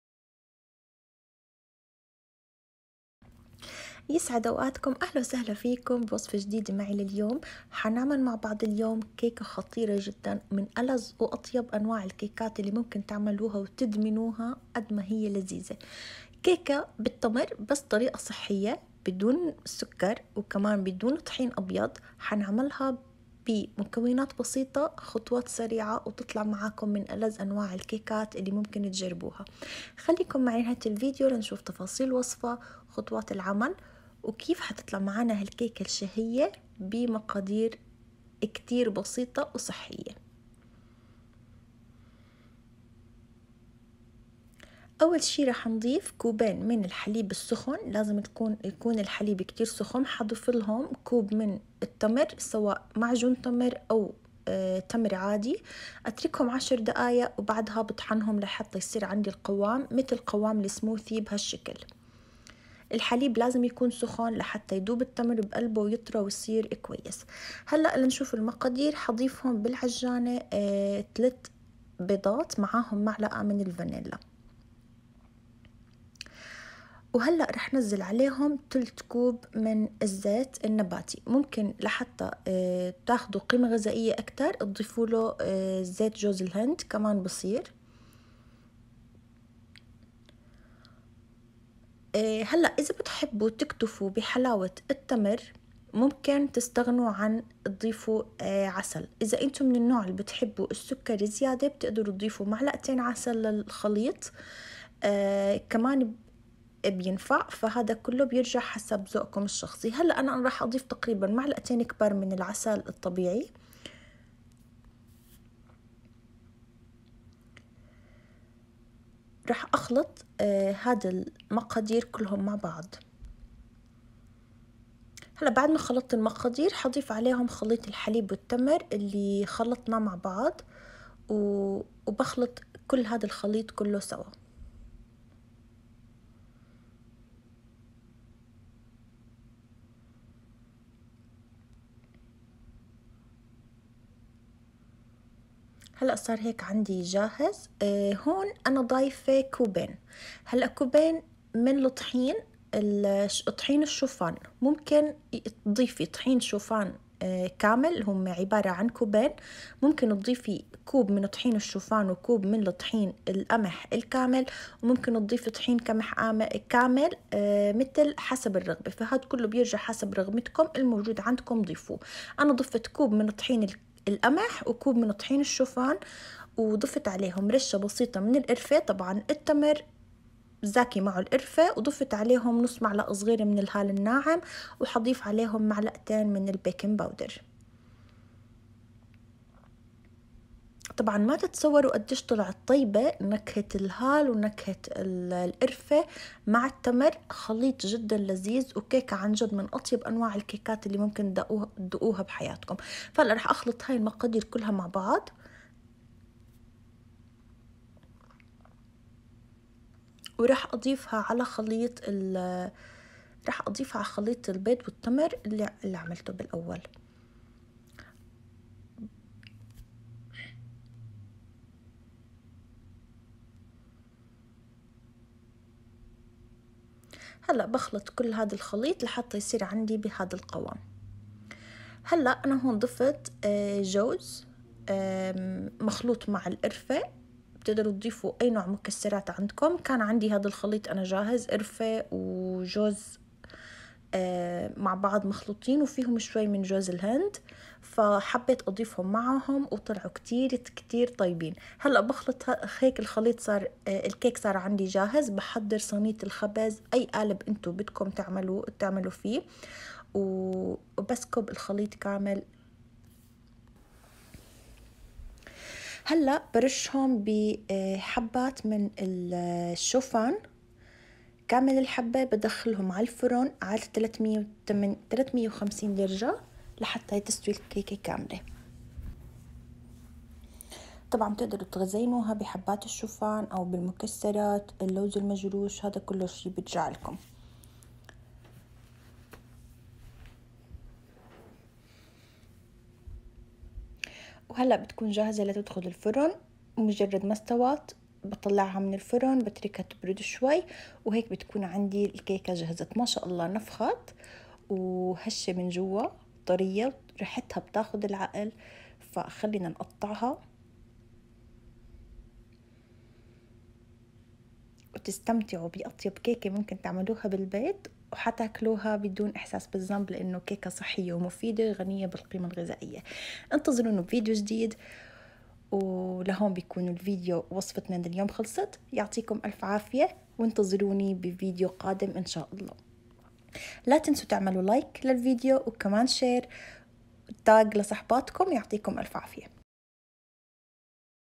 كيكه التمر الصحية بدون سكر وبدون طحين مش حتصدقًو ابدا انها كيكة صحيه طعمها خيال healthy date cake
كيكه التمر الصحيه
ثلاث بيضات  مع معلقه فانيلا
كوبين حليب ساخن لدرجه الغليان
كوب تمر معجون او تمر عادي
ثلث كوب زيت نباتي. ( ممكن استخدام زيت جوز الهند )
كوبين طحين شوفان  او طحين قمح كامل او كوب طحين شوفان وكوب طحين قمح
معلقتين كبار عسل معلقتين صغار بيكنج باودر نصف معلقه صغيره قرفةنصف معلقه صغيره هال
جوز او اي نوع مكسرات حسب الرغبه
بضيف الحليب السخن على التمر وبتركه عشر دقايق بعدها بطحنه كتير ليصير متل لسموثي
بخفق البيض والفانيلا والزيت. ومعلقتين العسل وبضيف عليهم خليط الحليب والتمر
بضيف جميع مواد الجافه وبسكبهم ع خليط البيض والتمر  بحطهم بصينيه الخبز وبحط ع وجه شوفان لتزين  بخبزهم ٣٥٠ لتستوي الكيكه تماما

‏Healthy date cake
3 eggs with 1 teaspoon vanilla
1/3 cup vegetables oil or coconut oil
2 cup hot Low fat milk
1 cup of  Date paste
2 tablespoon honey
2 cups of oats flour or  Wheat flour or a mixture between wheat flour and oat flour
2 teaspoon Baking powder
1/2 teaspoon cinnamon and 1/2 teaspoon  Cardamom
Walnut optional
 Mix the hot milk with the dates paste and leave them for ten minutes then mix them to become creamy mixture eggs vanilla and vegetable oil and honey then add the milk mixture and dates mix dry materials and add to the previous mixture then add the nut and then pour the mixture in a baking bowl and bake at a temperature of 350 F